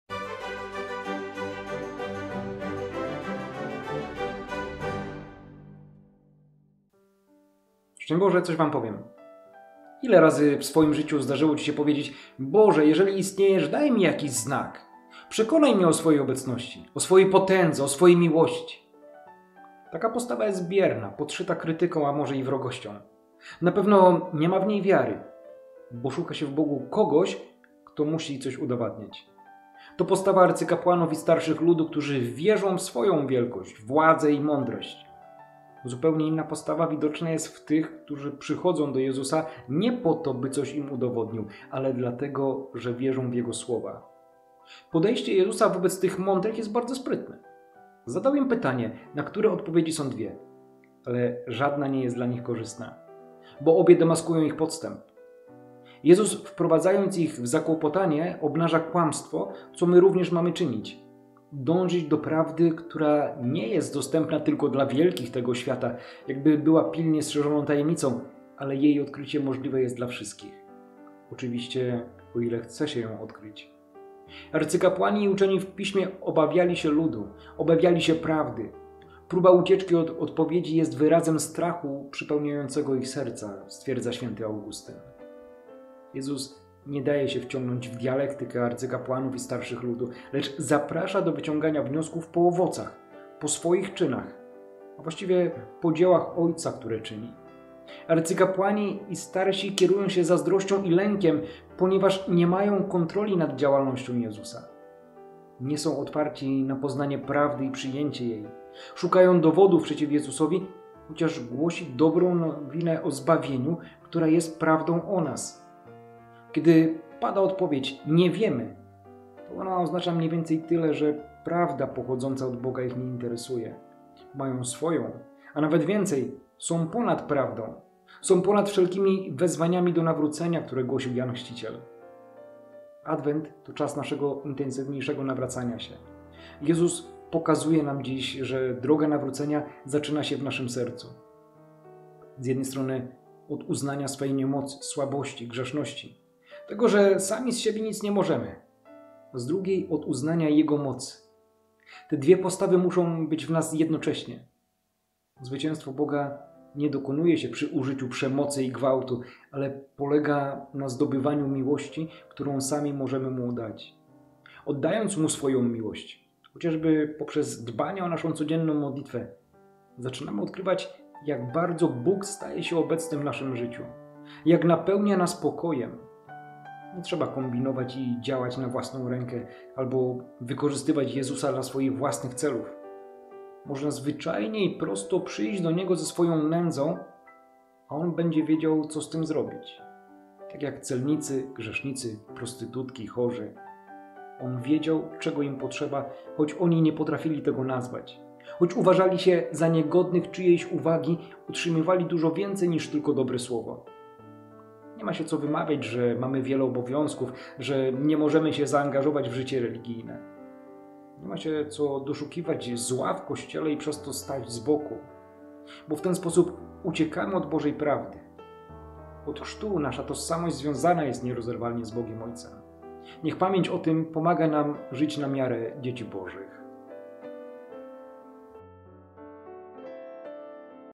coswampowiem Mt 21, 23-27 || 12 grudnia 2022
Ile razy w swoim życiu zdarzyło Ci się powiedzieć: Boże, jeżeli istniejesz, daj mi jakiś znak? Przekonaj mnie o swojej obecności, o swojej potędze, o swojej miłości.